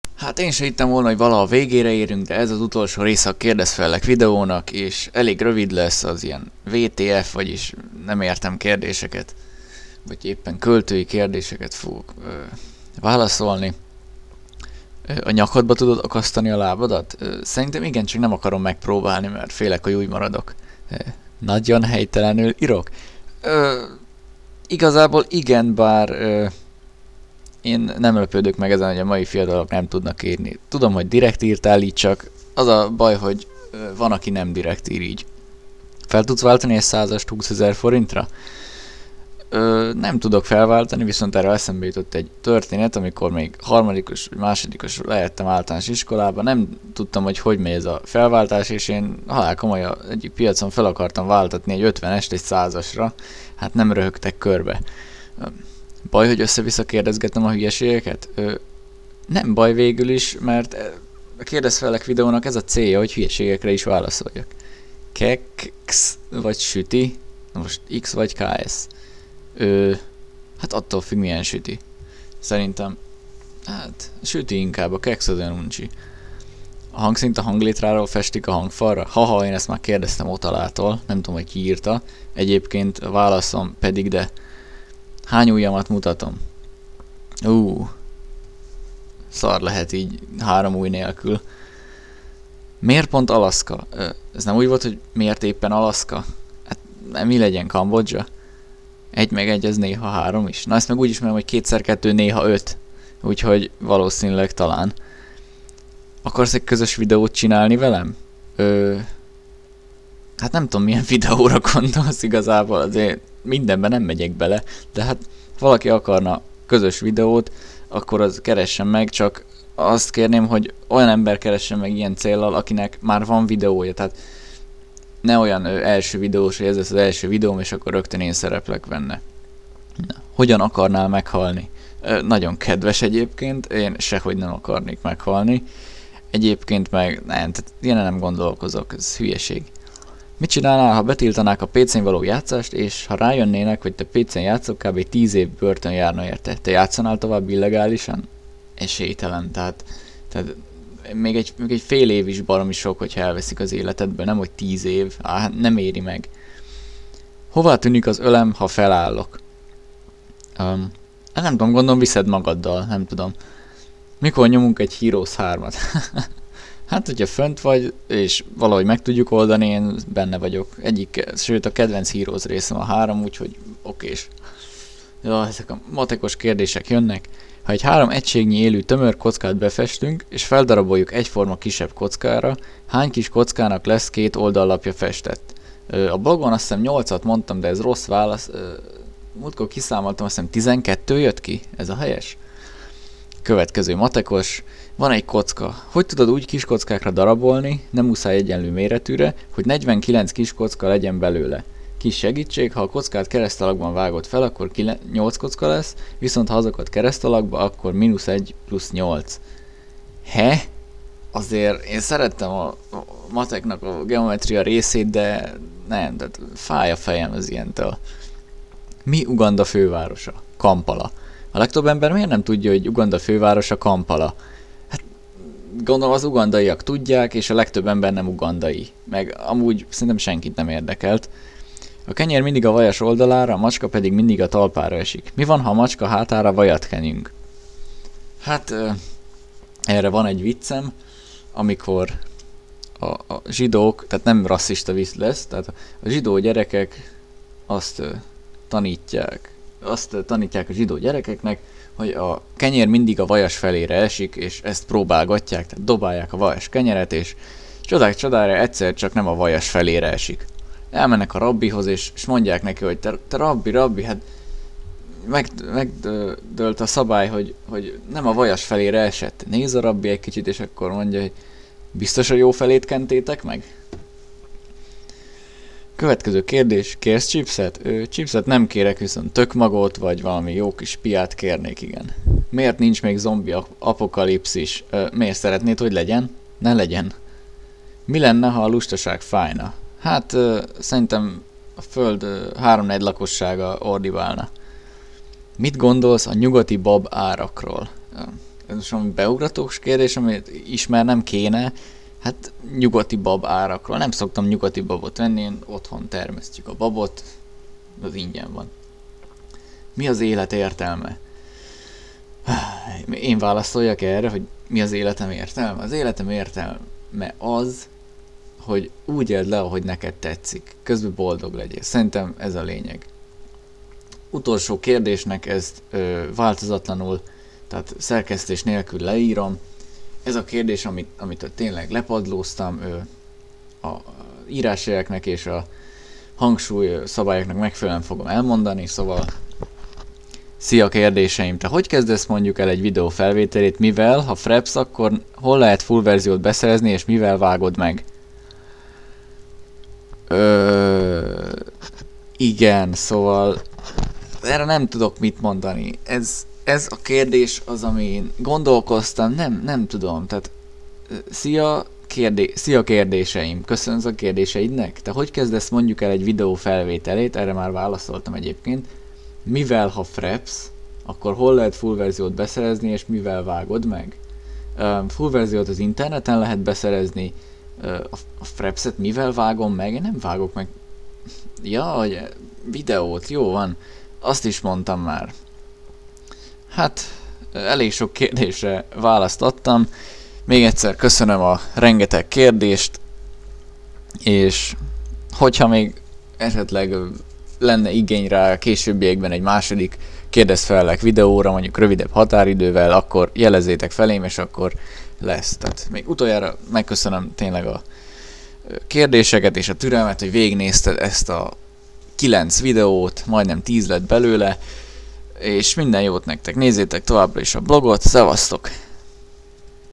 RockerLaLee: Kérdezz - Felelek (9/9)
A válaszvideó-sorozat kilencedik és egyben befejező része, amelyben a maradék 'WTF??' kérdésekre adok választ, ha tudok.